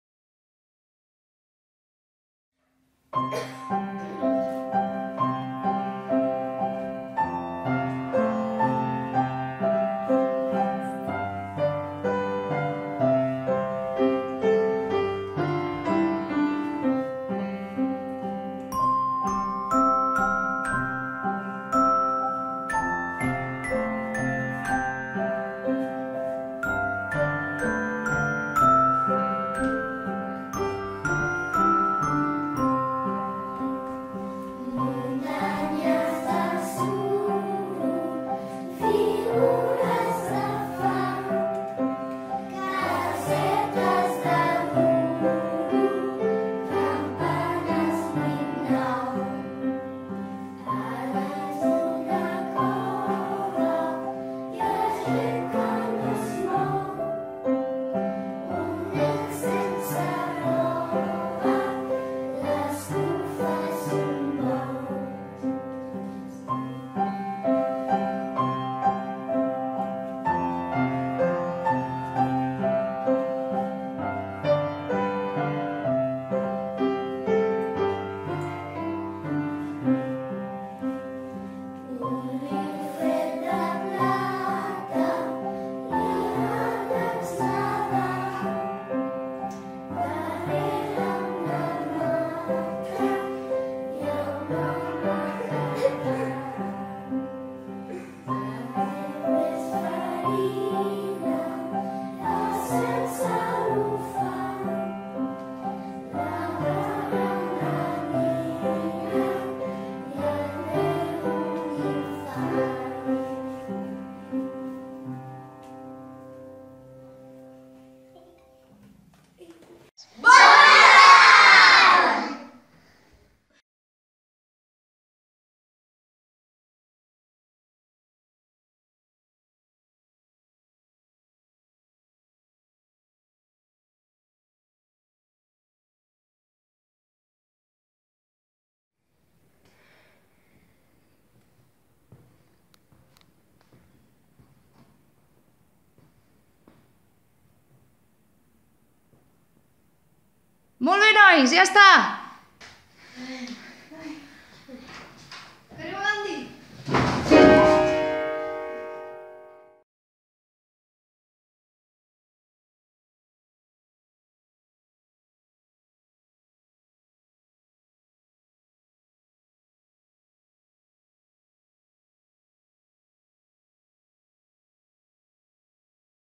El pessebre a l'escola / Desembre 2016
Felicitació de Nadal de l'Escola Municipal de Música
de Palau-solità i Plegamans